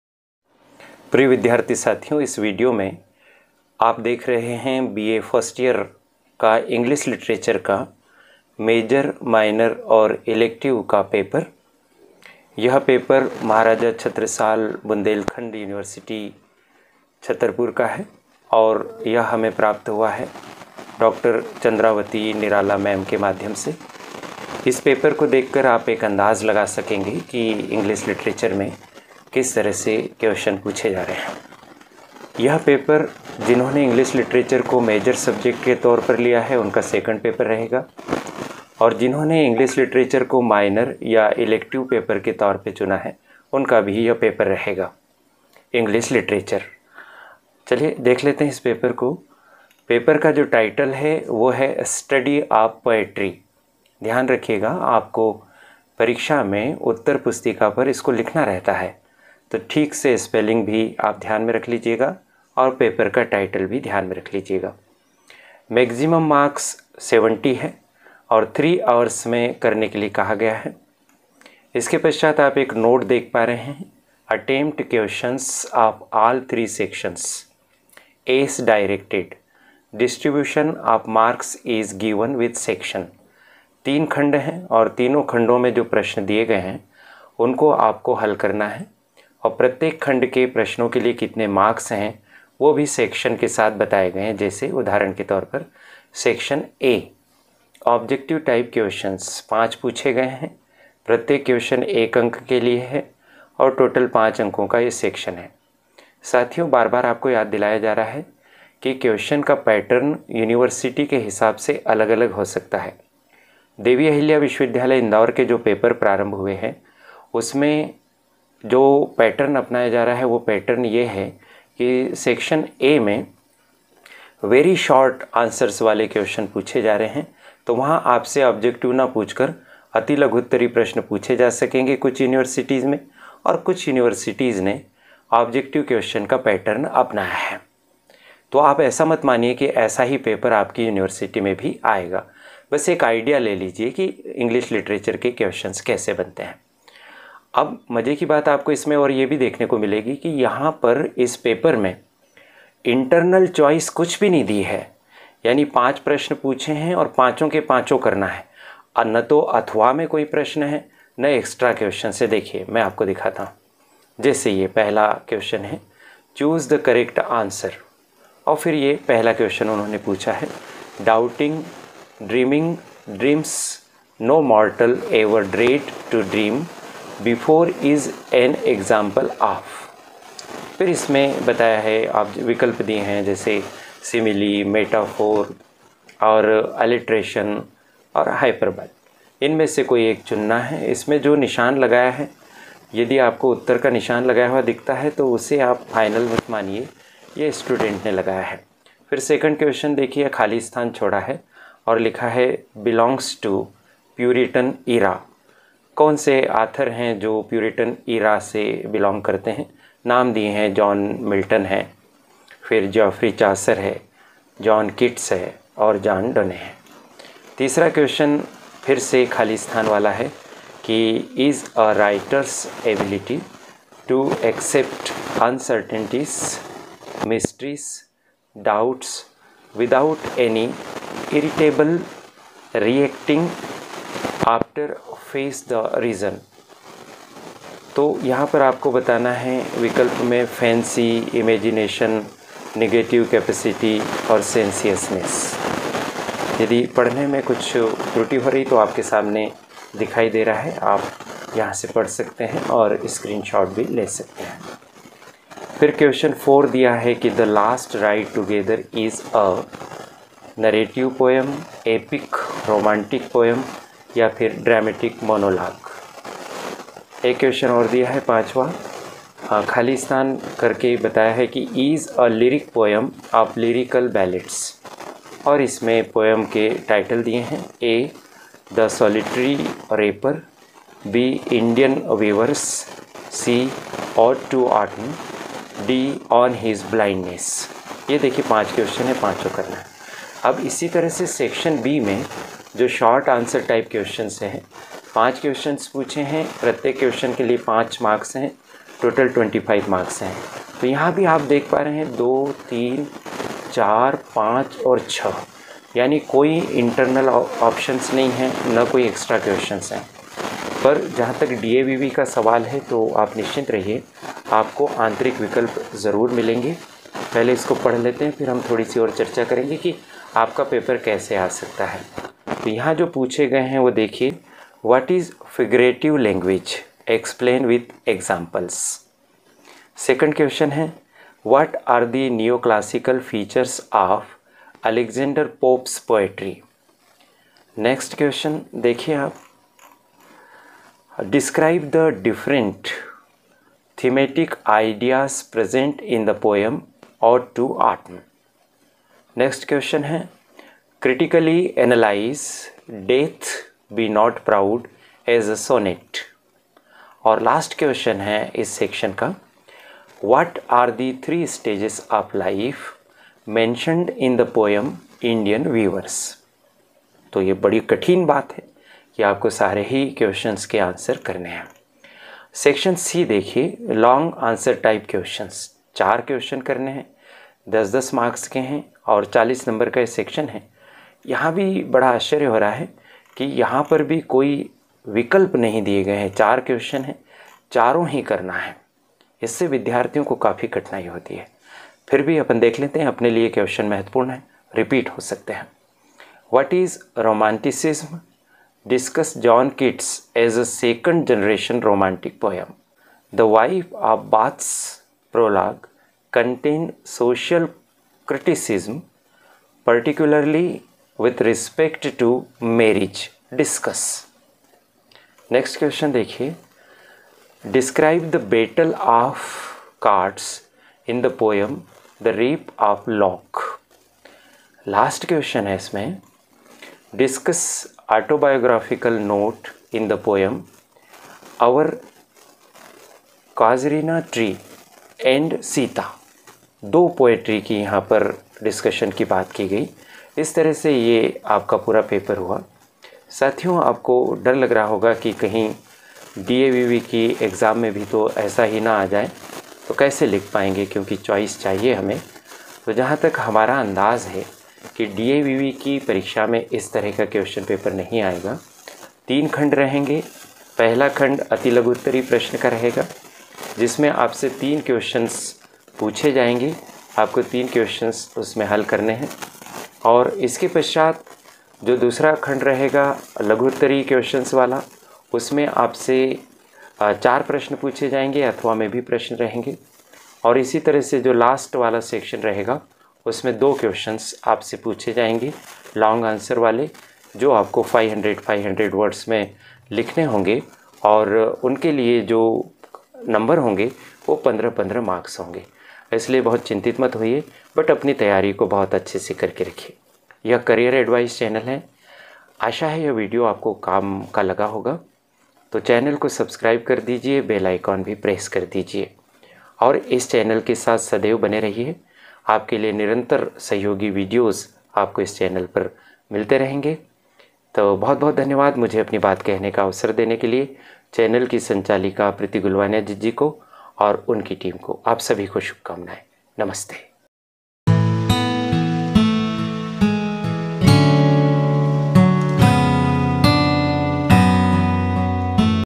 B.A. FIRST YEAR ENGLISH LITERATURE STUDY OF POETRY का पेपर देखिये. मेजर, माइनर, इलेक्टिव
प्रिय विद्यार्थी साथियो,
इस वीडियो में आपको यह बताया जा रहा है कि  राष्ट्रीय शिक्षा नीति के अंतर्गत बी ए प्रथम वर्ष के अंग्रेजी साहित्य  विषय के मेजर सेकंड पेपर, माइनर पेपर और इलेक्टिव पेपर स्टडी ऑफ़ पोएट्री  का पेपर किस प्रकार का आ रहा है. यह पेपर महाराजा छत्रसाल बुंदेलखंड यूनिवर्सिटी का है... इस पेपर को देखकर आपको आइडिया मिलेगा कि आपकी यूनिवर्सिटी में पेपर किस प्रकार का आएगा.. साथ ही इस पेपर में पूछे गये प्रश्नों में से भी प्रश्न आपसे पूछे जा सकते हैं.